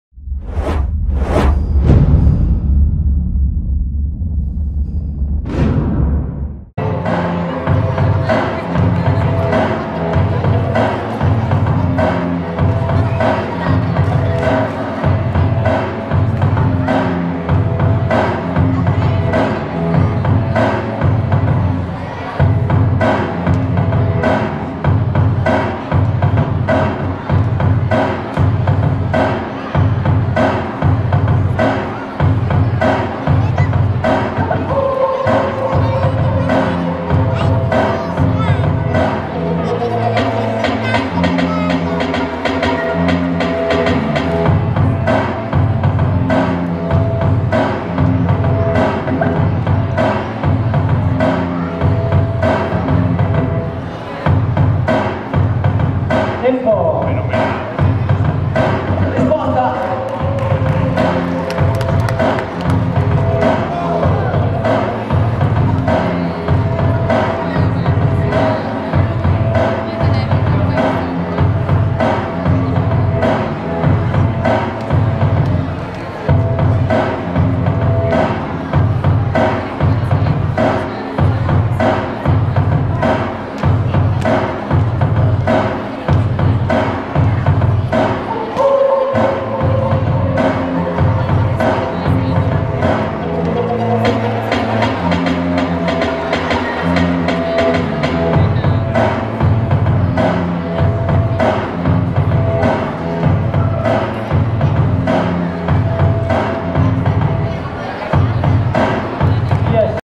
Lindy vs Camomilla [semiFINALE] Hip-Hop Under 11 | Conè Contest V4
Cone' Contest V4 by Street Warriors Crew (25/10/2014)

Hip-Hop Contest

Semifinale Beginner Under 11

Winner: Lindy
Camomilla 3°Posto